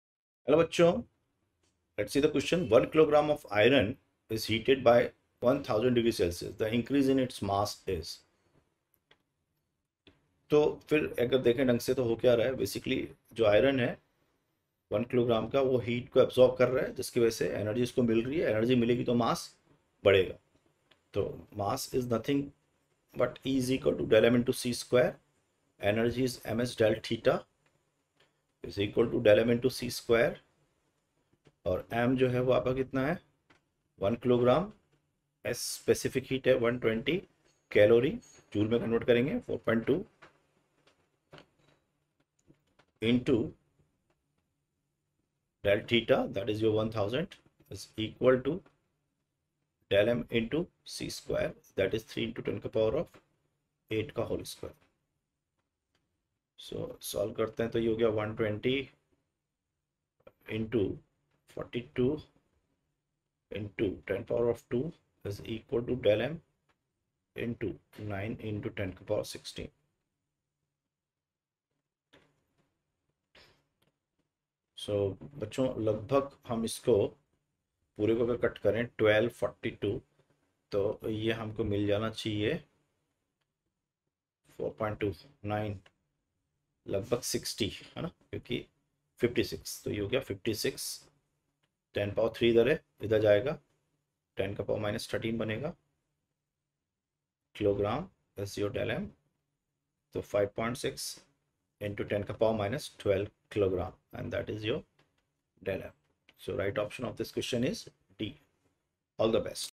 1 kg of iron (specific heat 120 calkg^-1C^-1. is heated by 1000^∘C. The increase in its mass is ...
1  kg of iron (specific heat 120 calkg^-1C^-1. is heated by 1000^∘C. The increase in its mass is
a. Zero
b. 5.6 × 10^-8Kg
c. 5.6 × 10^-16Kg
d. 5.6 × 10^-12Kg
θ